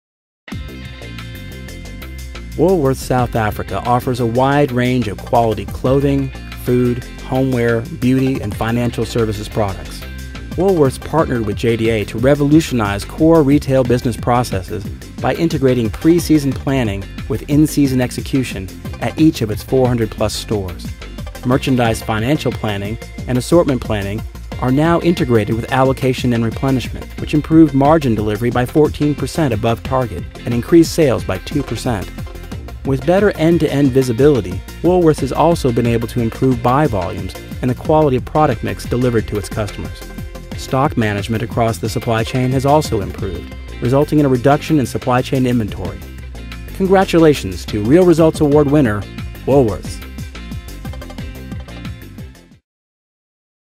Woolworths South Africa Wins Supply Chain Award
Congratulations to Woolworths South Africa for winning the 2012 JDA Software Real Results Award for excellence in supply chain management.